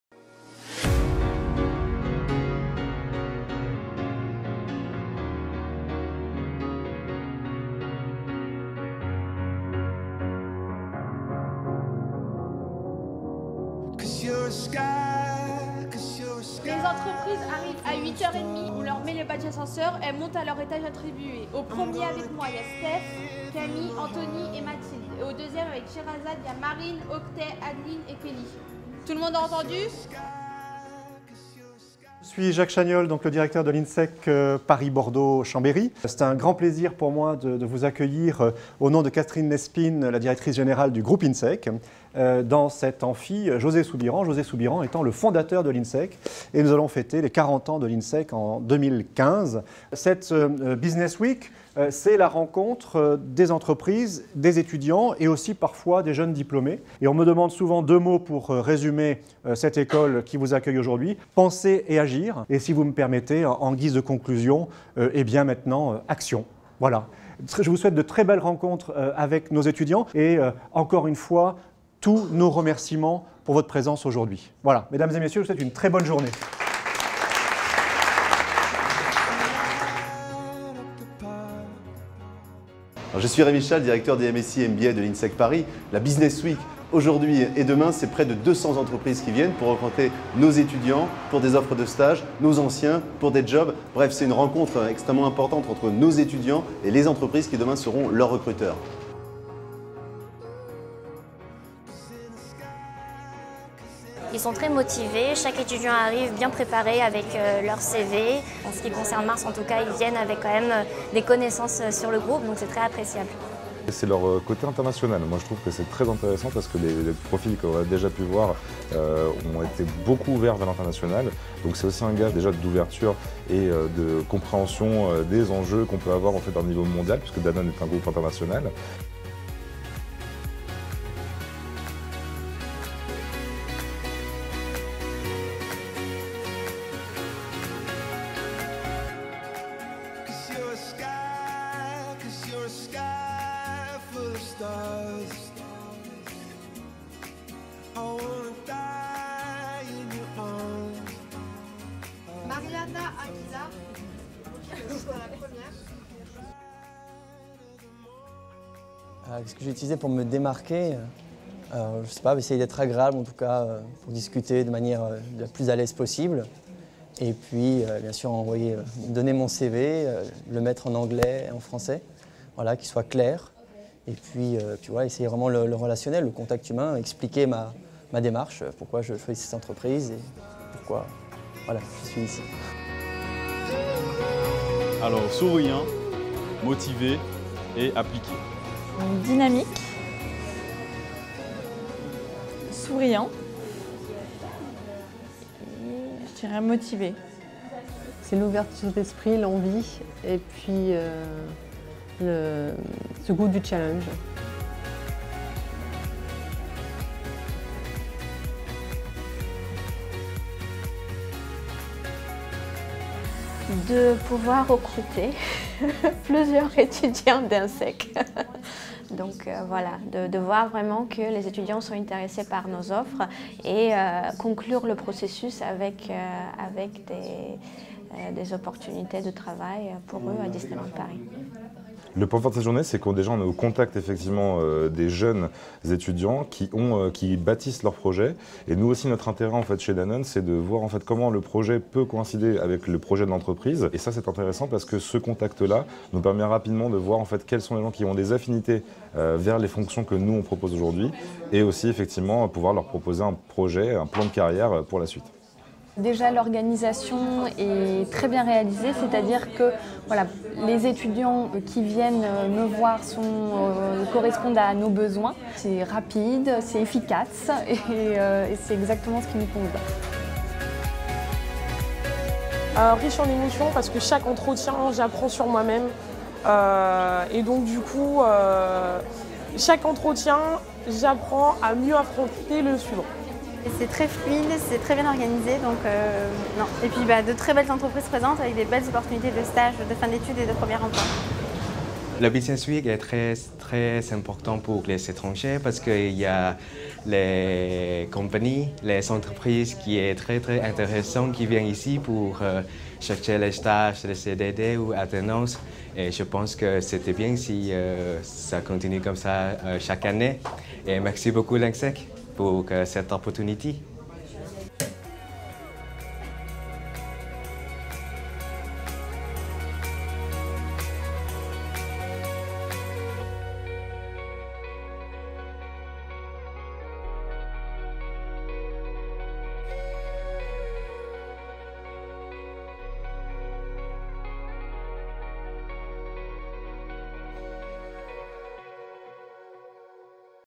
BUSINESS WEEK INSEEC PARIS - 2014
Découvrez le Forum Entreprises des écoles du groupe INSEEC à Paris : Insignis Business School, MSc & MBA INSEEC, Bachelor INSEEC, BBA International INSEEC, Sup de Pub, Sup Career.